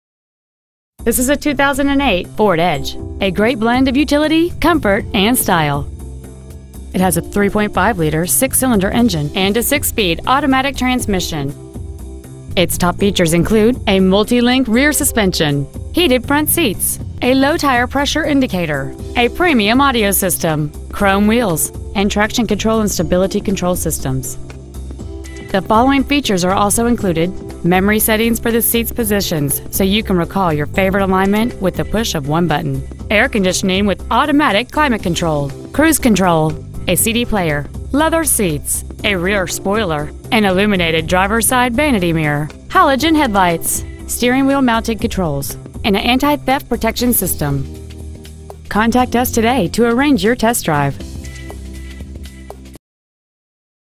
2008 Ford Edge Dallas GA 30157
Year : 2008
 Make : Ford
 Model : Edge
 Engine : Gas V6 3.5L/213
 Trans . : Automatic
 Exterior : Black Clearcoat
 Miles : 76,315
 Interior : Camel w/Leather-Trimmed Bucket Seats
 HARDY CHEVY GMC
 1249 Charles Hardy Pkwy
 Dallas , GA 30157
 Pre-Owned 2008 Ford Edge Dallas GA